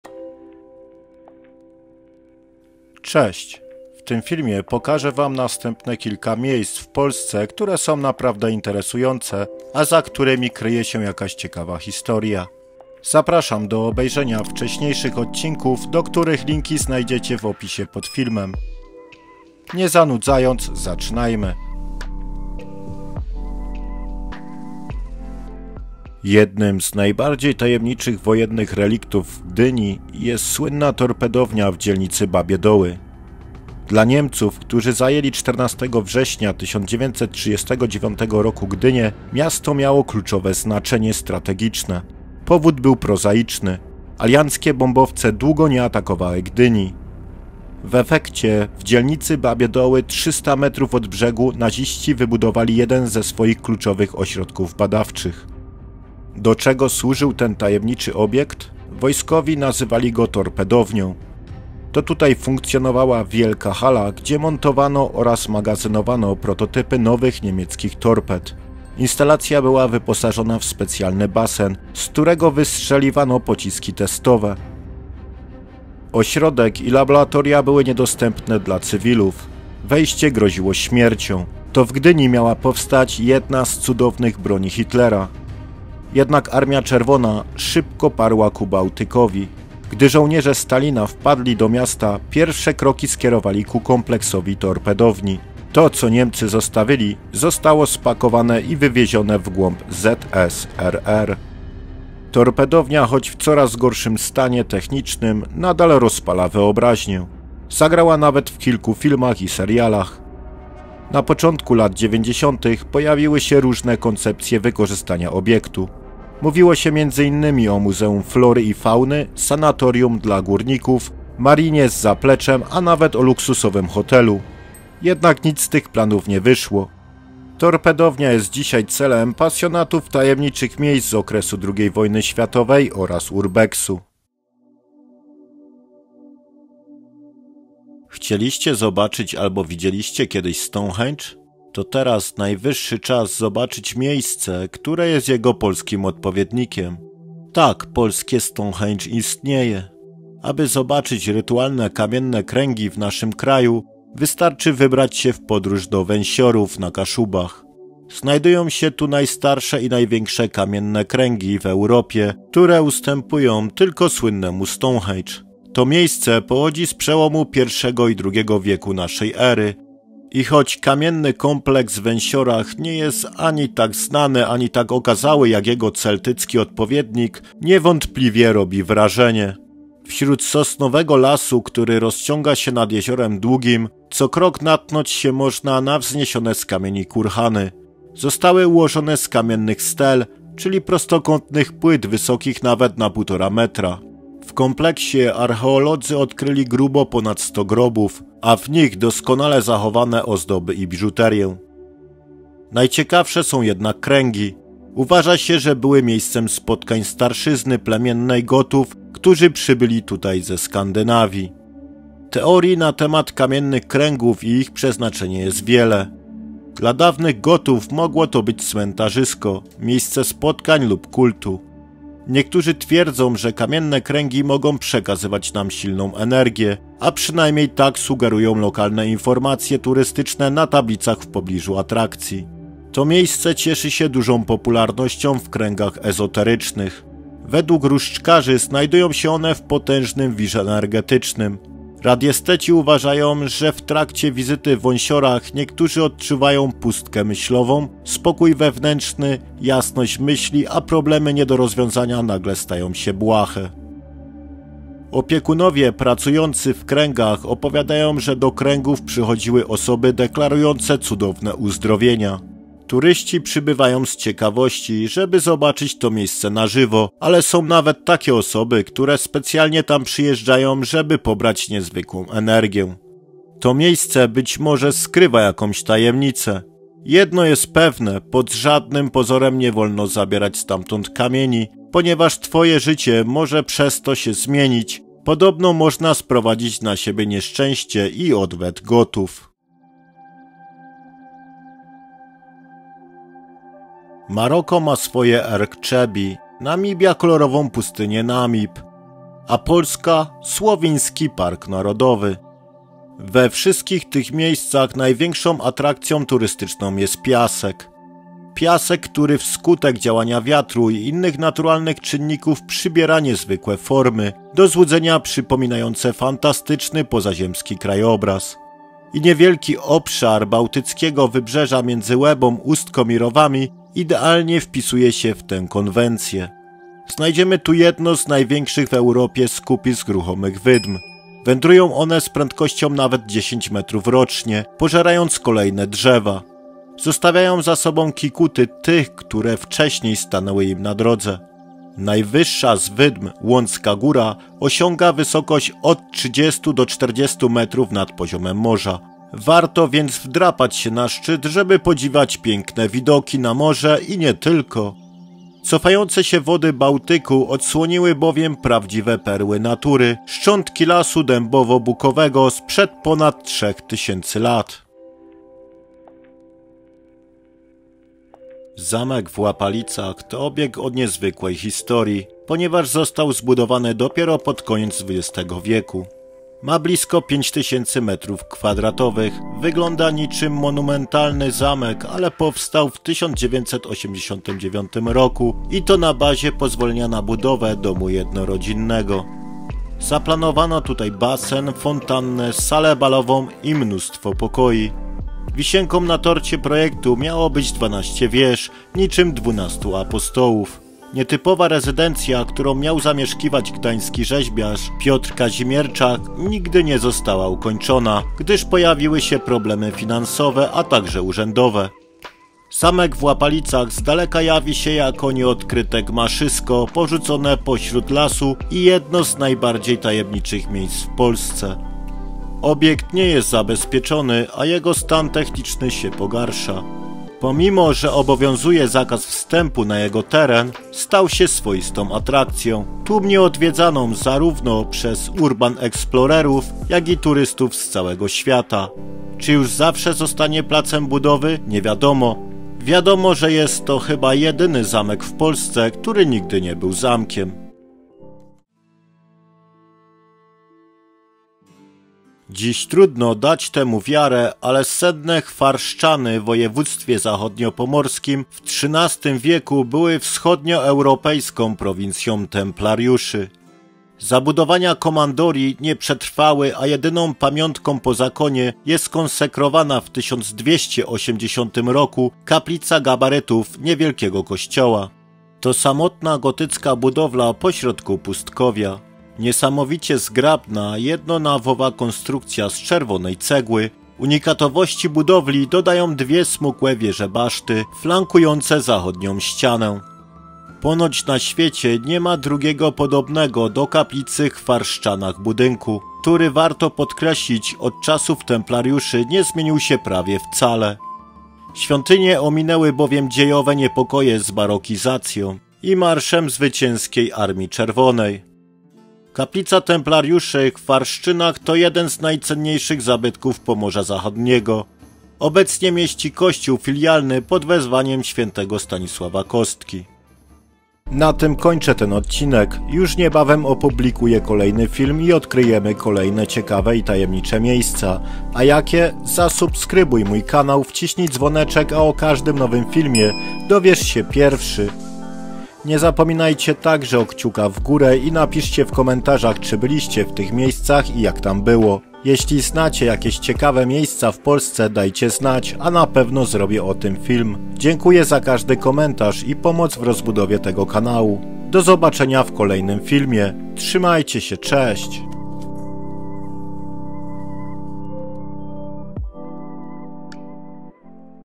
ODKRYJ POLSKĘ Na Nowo
Odkryj z nami Polskę na nowo!

0:00 Wstęp
0:23 Torpedownia w Gdyni
2:16 Kamienne Kręgi w Węsiorach
4:51 Słowiński Park Narodowy
6:06 Zamek w Łapalicach
7:31 Kaplica Templariuszy Chwarszczany
8:40 Zakończenie